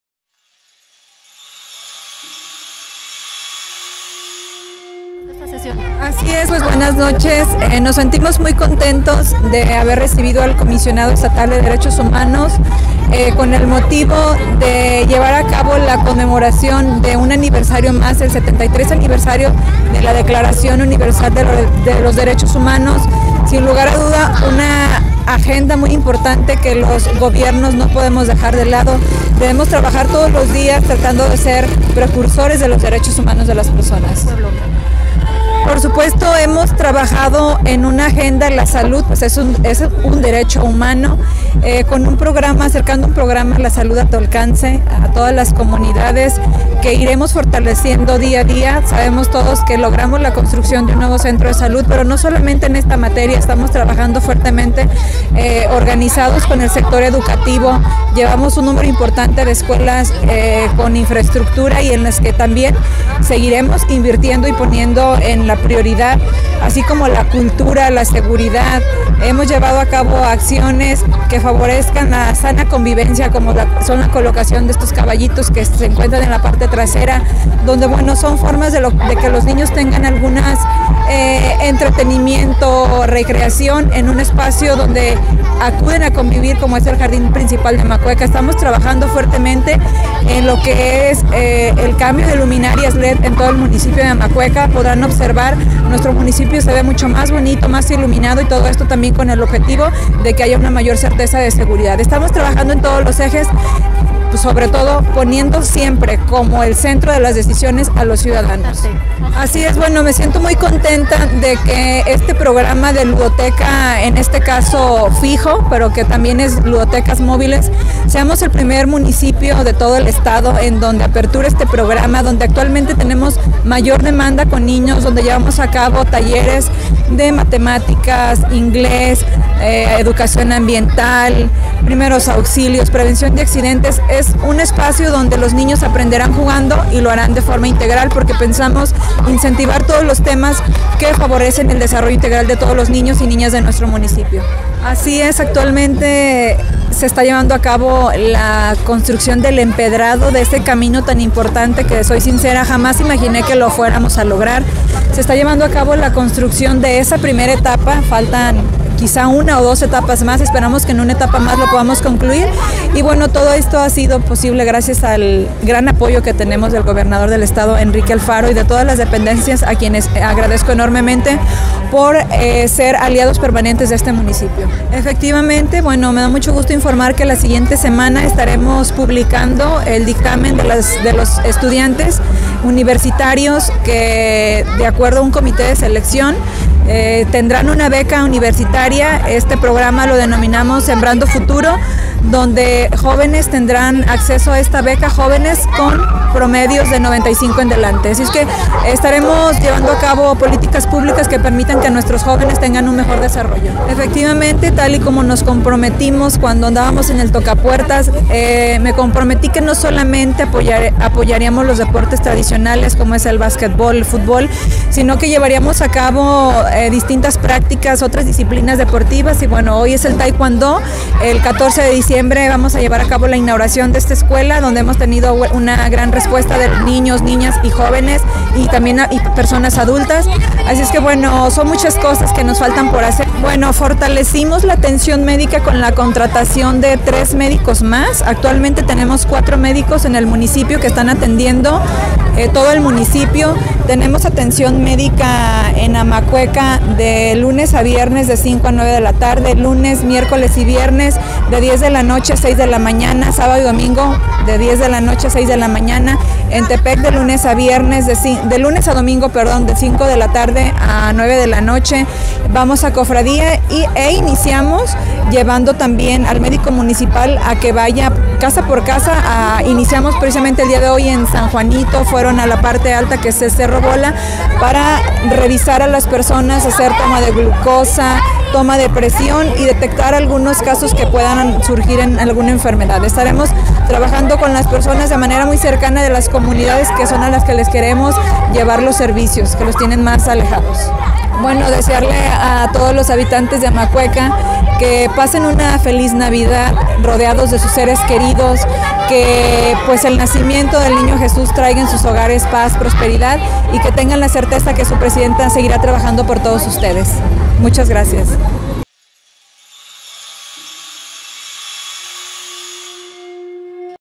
La salud es la prioridad en Amacueca
Entrevistamos a la presidenta municipal de Amacueca, Luz Elvira Durán Valenzuela.